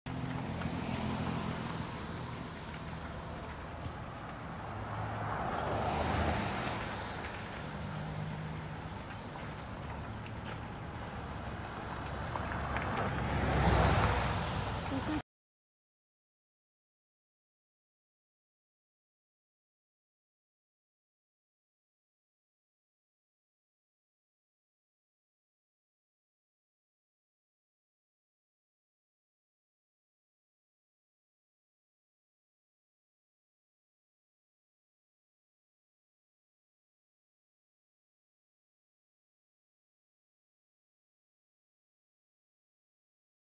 Fiery fatal crash closes Wisconsin interstate
At least two people are dead after a semi crashed and exploded on Interstate 94 in southeastern Wisconsin (June 19)